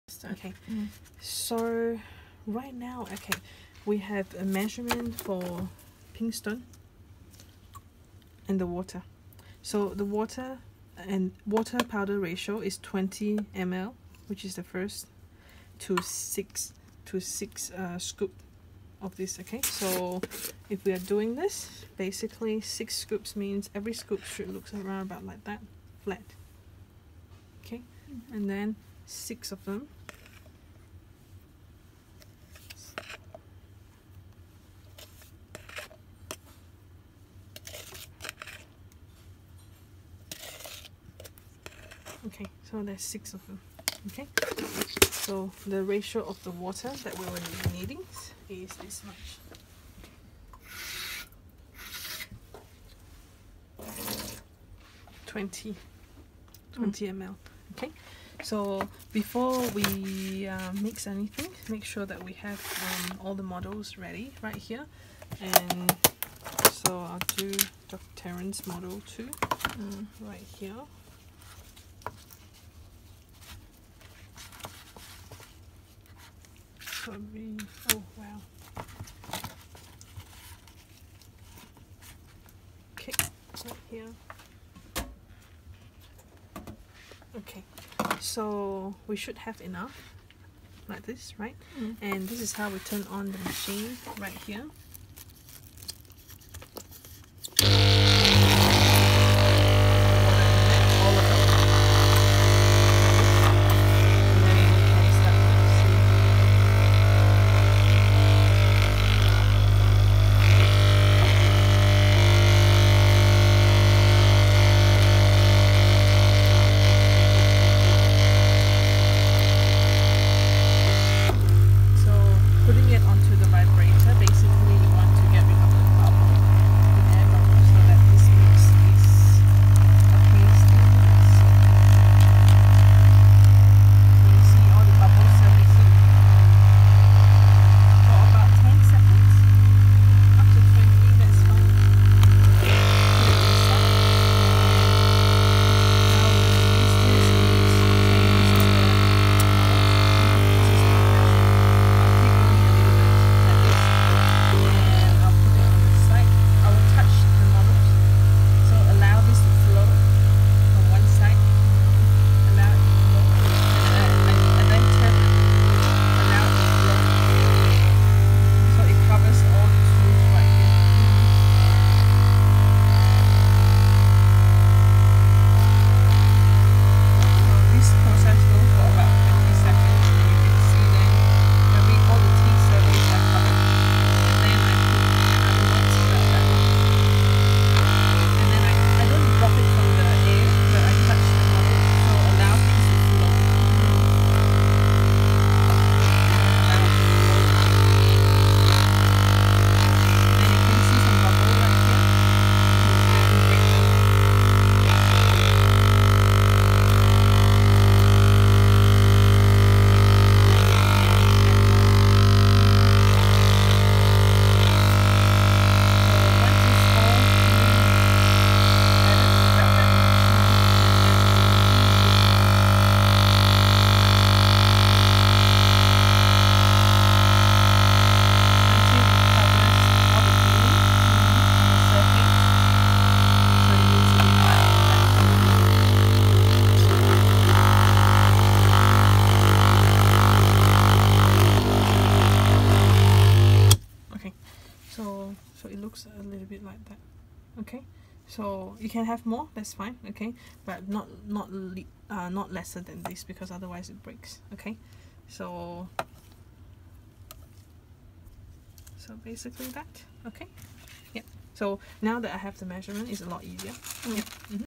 How to pour up model with pink die ston
6 scoop of powder to 20ml water, mix the powder and water until they get homogenous and vibrate to surface and break all bubbles.  When pouring model, take a small amount of stone mixture and let it flow down into the impression, rotate the mixture until it cover the first layer of the model, then add a second lot of mixture and follow the same rotation.
Continue until all fitting surface of the alginate is covered.